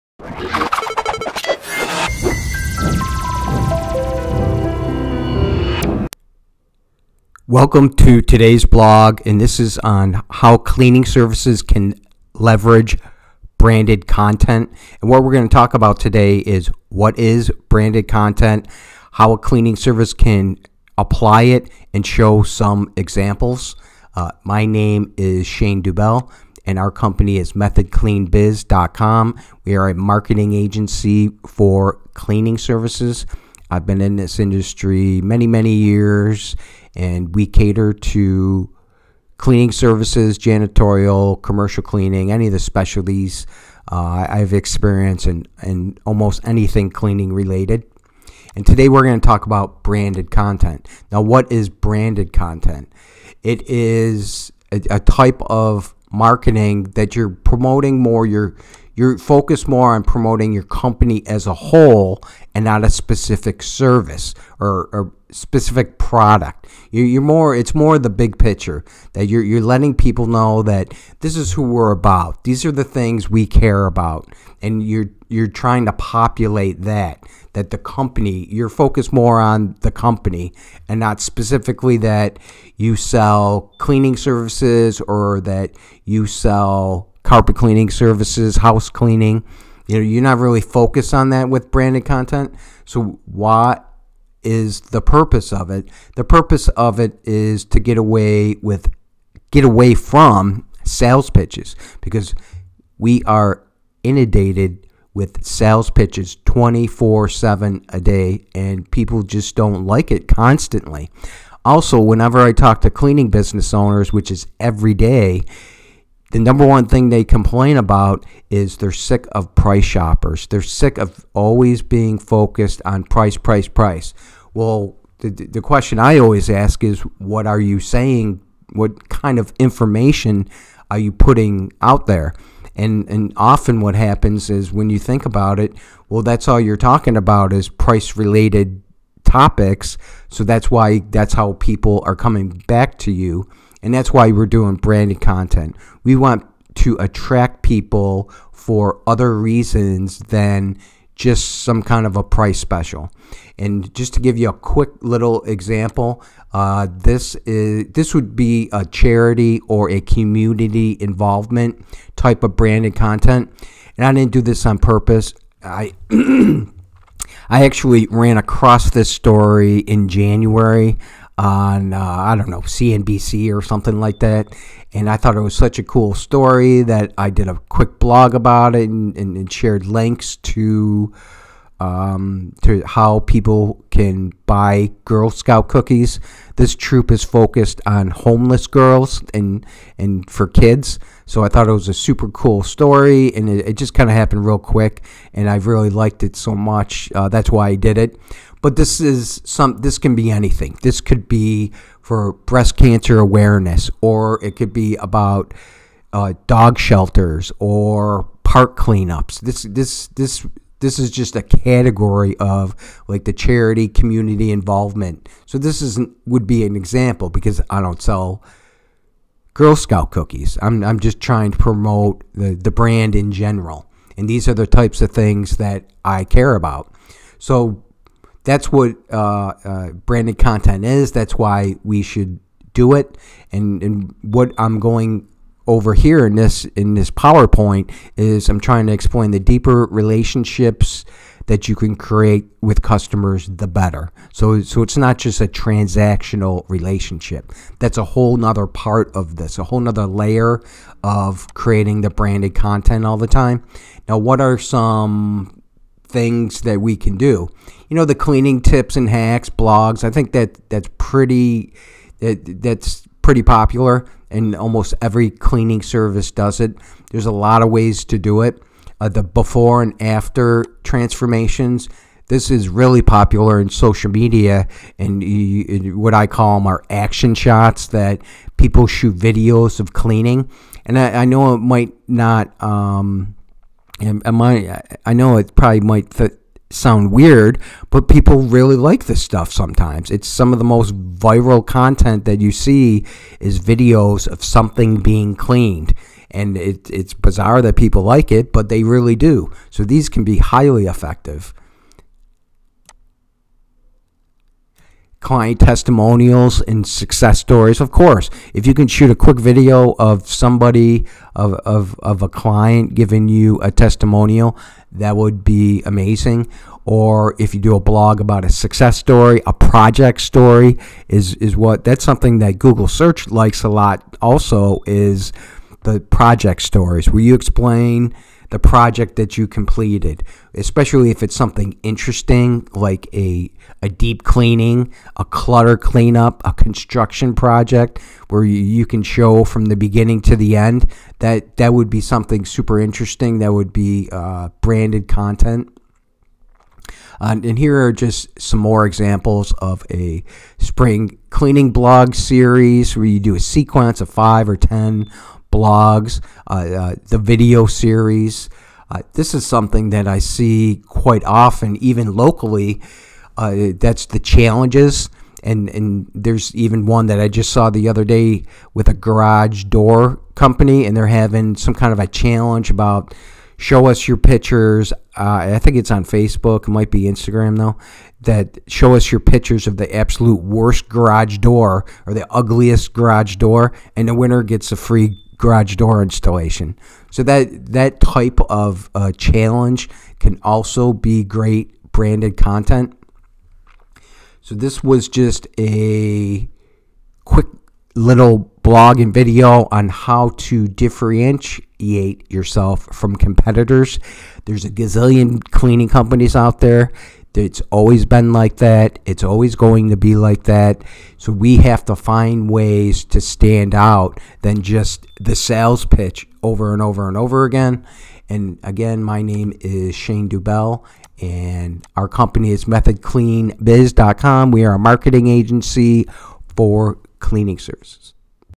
How Cleaning Services Can Leverage Branded Content
Join us as we explore the many reasons why cleaning services should embrace branded content. By crafting content that entertains, informs, and resonates emotionally, cleaning companies can break through the clutter and captivate their target audience like never before.

Discover how branded content offers a non-intrusive platform for cleaning services to showcase their expertise and knowledge. Gone are the days of relentless sales pitches – instead, branded content enables companies to demonstrate value and credibility in a subtle yet impactful manner.

From sharing invaluable cleaning hacks to offering expert advice on home organization, branded content positions cleaning services as trusted advisors rather than mere service providers. Witness the power of before-and-after transformations as we highlight real-life success stories and inspire both homeowners and businesses alike.

Don't miss out on this invaluable resource for cleaning service providers looking to elevate their brand presence and forge meaningful connections with their audience.